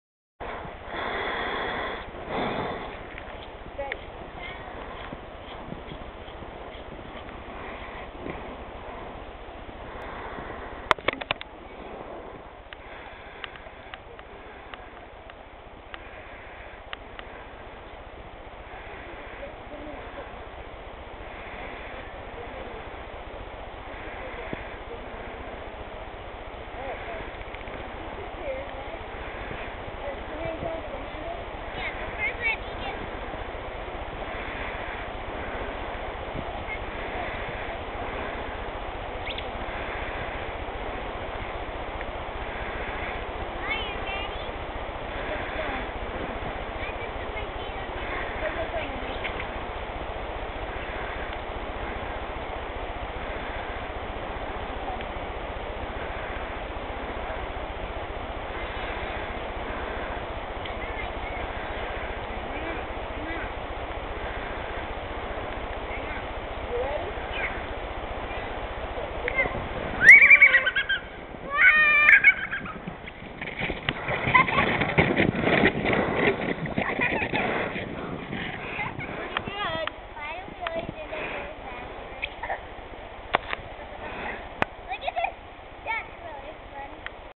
Gwyneth On Snow Mountain
Gwyneth on the mountain outside of Nachees, Wa